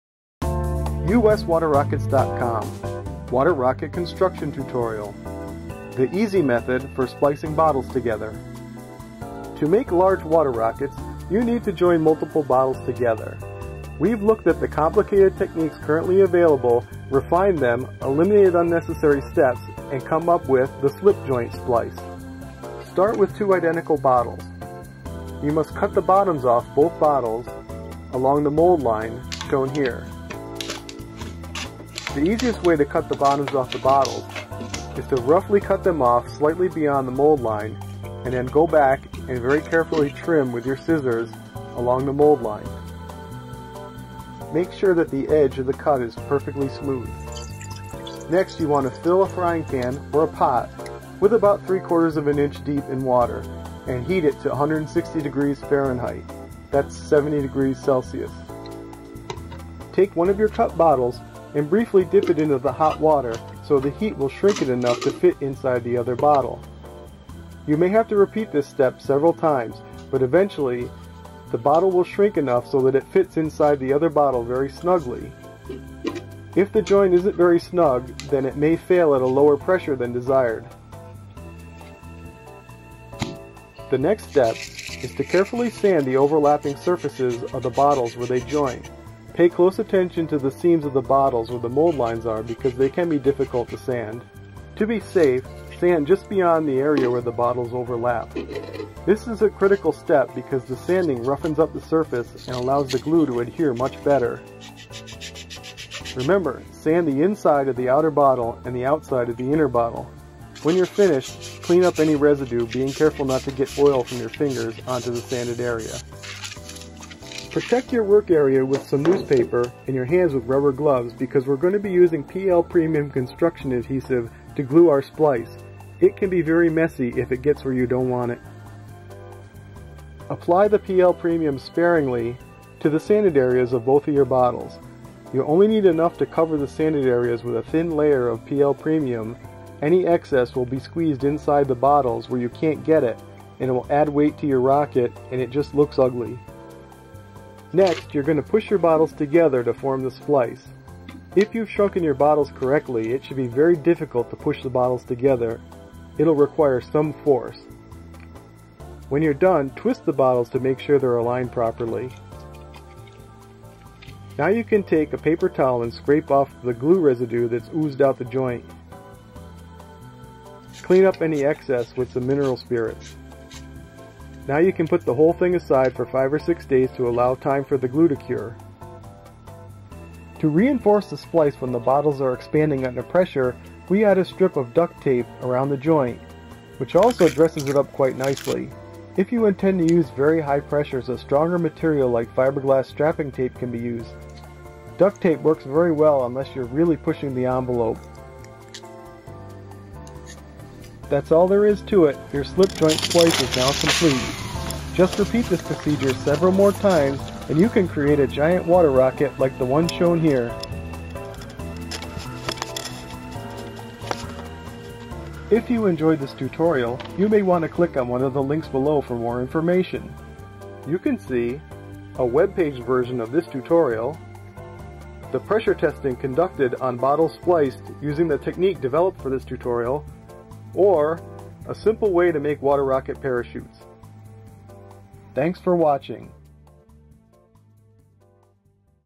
How To Make a Huge Water Rocket by Splicing Bottles Together!
Make huge water rockets by joining multiple bottles together using this tutorial.

If you want to see the Webpage version of this tutorial, please

This tutorial shows you how to join multiple bottles together to make a Huge Water Rocket using our Slip Joint Splicing Technique. This easy new method of joining bottles creates strong and attractive joints by reducing unnecessary and/or detrimental steps from the traditional splicing techniques commonly used.

Our testing shows these splices typically hold past the point where the other areas of the bottles become permanently damaged from the plastic stretching (which varies with different size bottles). On 500ml bottles, we have seen 195PSI sustained with this method.